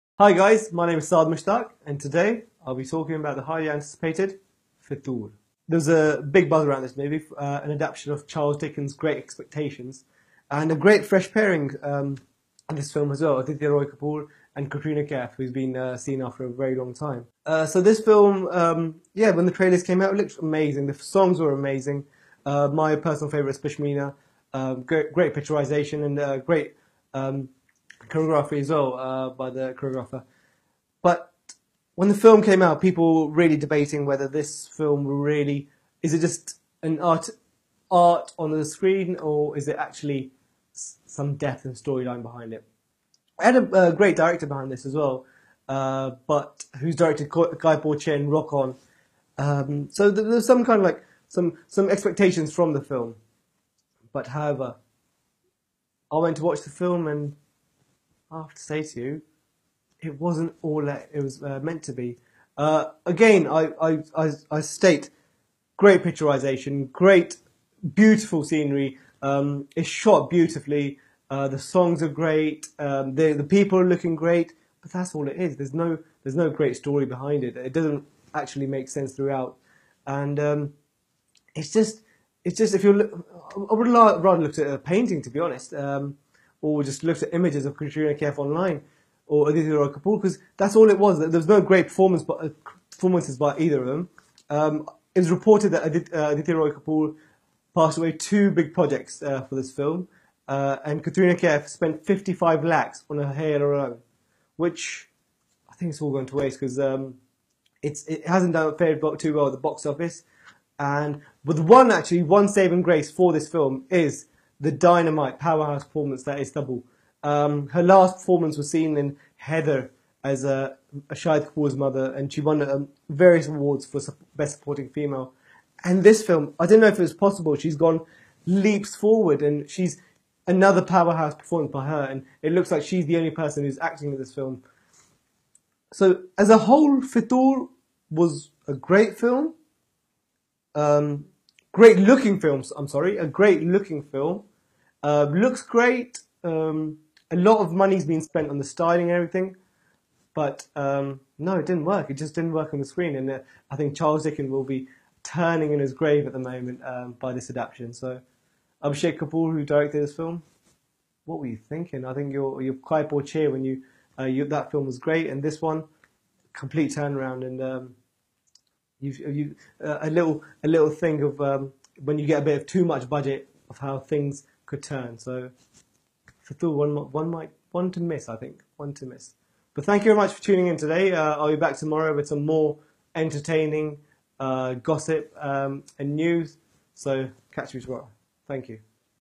Movie Review for Fitoor.
View our Movie review for Bollywood movie Fitoor. Saad will go through the film and tell you whether its a film worth watching.
‪#‎Fitoor ‪#‎vs‬ ‪#‎Bollywood ‪#‎Indian ‪#‎Movie‬ ‪#‎Blockbuster‬ ‪#‎Entertainment‬ ‪#‎Review‬ ‪#‎WeekendwithJay‬